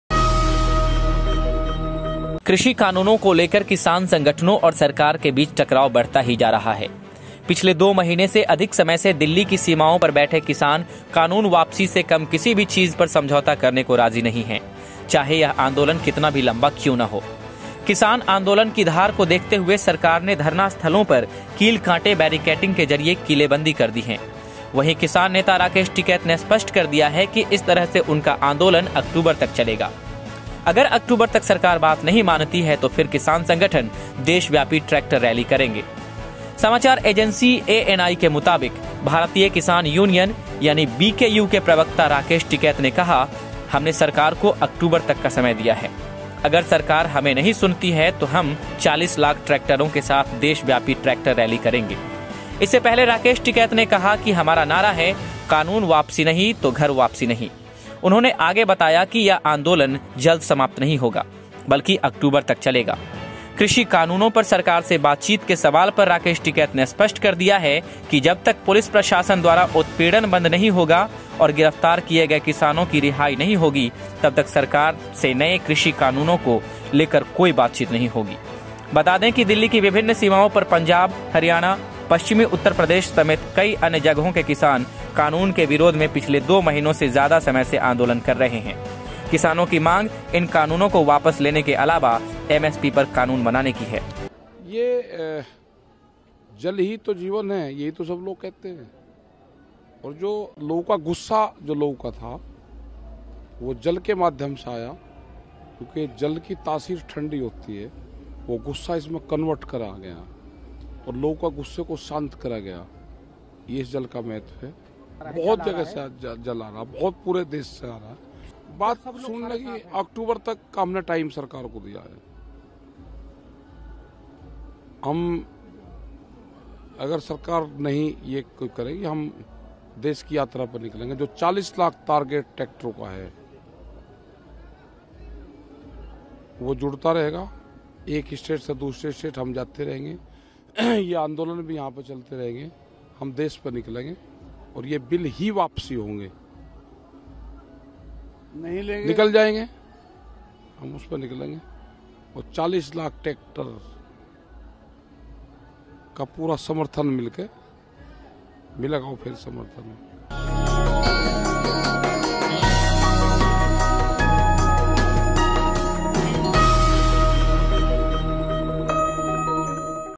किसानों का अल्टीमेटम - देशभर में 40 लाख ट्रैक्टरों की रैली निकालेंगे किसान
कृषि कानूनों को लेकर किसान संगठनों और सरकार के बीच टकराव बढ़ता ही जा रहा है। पिछले 2 महीने से अधिक समय से दिल्ली की सीमाओं पर बैठे किसान कानून वापसी से कम किसी भी चीज पर समझौता करने को राजी नहीं हैं, चाहे यह आंदलोन कितना भी लंबा क्यों न हो।